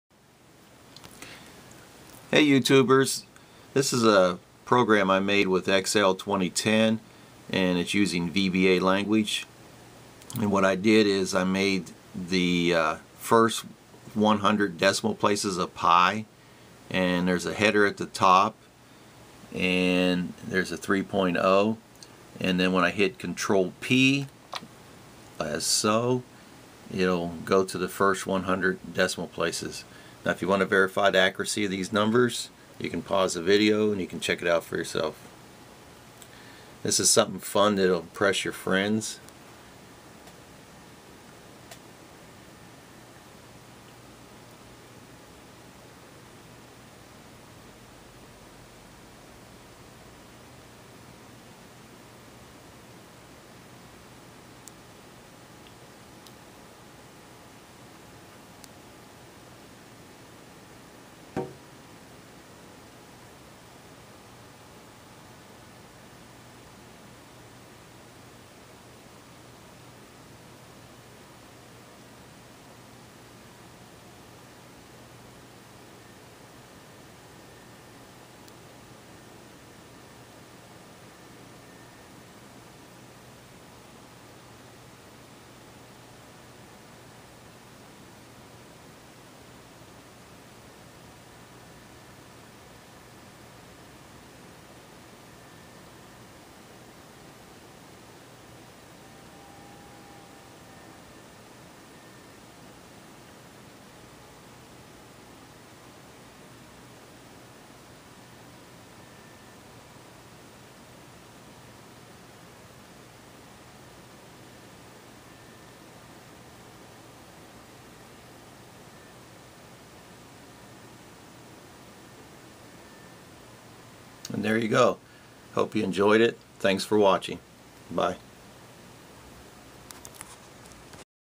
Pi in Excel
This was a fun project using VBA in Excel 2010 to generate the first 100 digits in Pi.